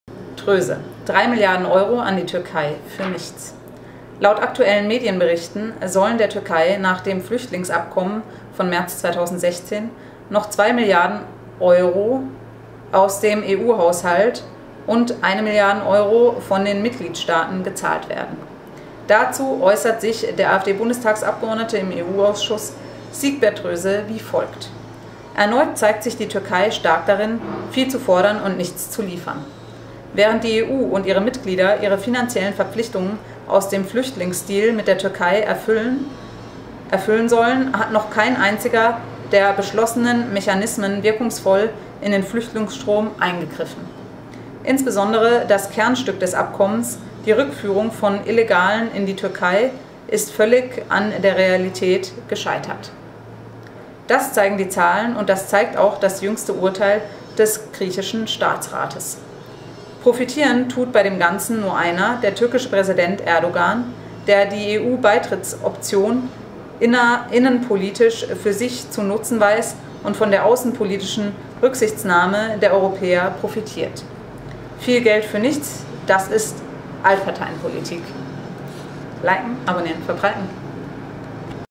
Droese: Drei Milliarden Euro an die Türkei für nichts
Laut aktuellen Medienberichten sollen der Türkei nach dem Flüchtlingsabkommen von März 2016 noch zwei Milliarden Euro aus dem EU-Haushalt und eine Milliarde Euro von den Mitgliedstaaten gezahlt werden.

Dazu äußert sich der AfD-Bundestagsabgeordnete im EU-Ausschuss, Siegbert Droese, wie folgt:

„Erneut zeigt sich die Türkei stark darin, viel zu fordern und nichts zu liefern: Während die EU und ihre Mitglieder ihre finanziellen Verpflichtungen aus dem Flüchtlingsdeal mit der Türkei erfüllen sollen, hat noch kein einziger der beschlossenen Mechanismen wirkungsvoll in den Flüchtlingsstrom eingegriffen. Insbesondere das Kernstück des Abkommens, die Rückführung von Illegalen in die Türkei, ist völlig an der Realität gescheitert. Das zeigen die Zahlen und das zeigt auch das jüngste Urteil des griechischen Staatsrates. Profitieren tut bei dem Ganzen nur einer: der türkische Präsident Erdogan, der die EU-Beitrittsoption innenpolitisch für sich zu nutzen weiß und von der außenpolitischen Rücksichtnahme der Europäer profitiert. Viel Geld für nichts - das ist Altparteienpolitik.“